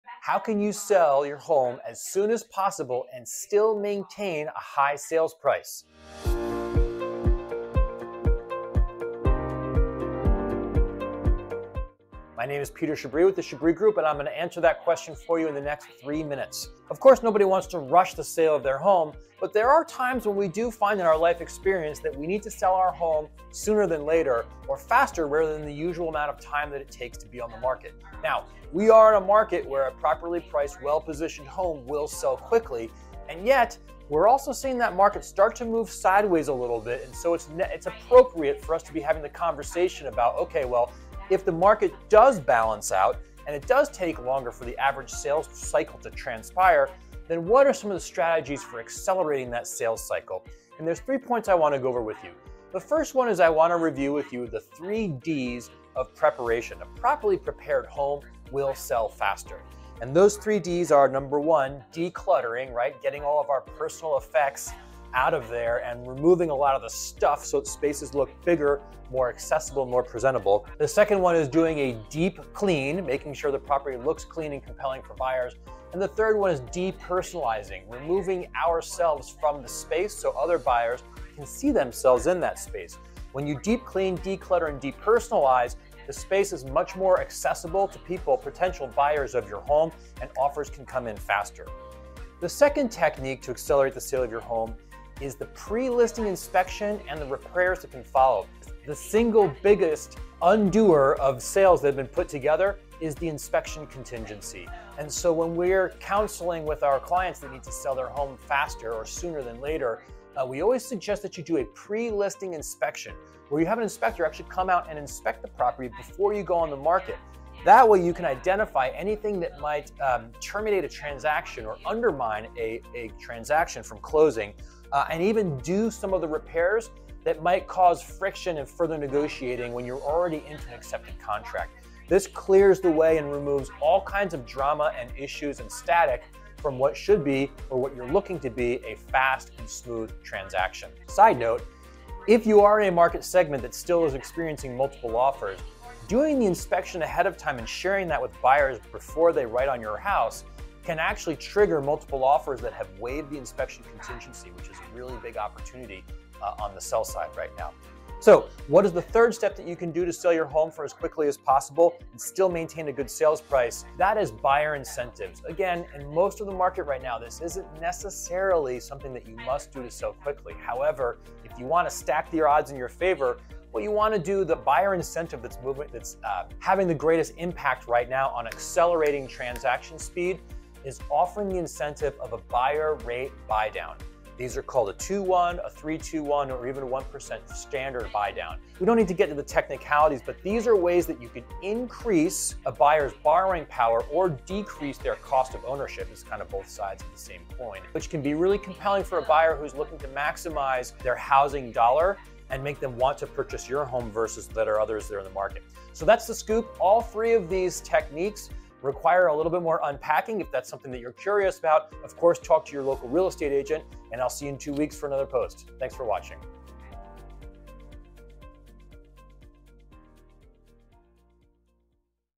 Your Simple Guide To Selling Your Home ASAP - Peter Chabris
Even when you have all the time in the world, selling your home can be a pain. If you’re in a position where you need to sell as soon as possible, the situation just gets worse. What can you do to sell quickly without sacrificing your sales price? Fortunately, I have a ton of experience with this, so I’m sharing three easy ways you can sell quickly and still get top dollar. To hear
what they are, check out this video.

Peter Chabris
The Chabris Group

CHAPTERS:
0:00 - Introduction
0:52 - Preparing Your Home
1:31 - Take Care of Repairs
2:39 - Offer Incentives to Buyers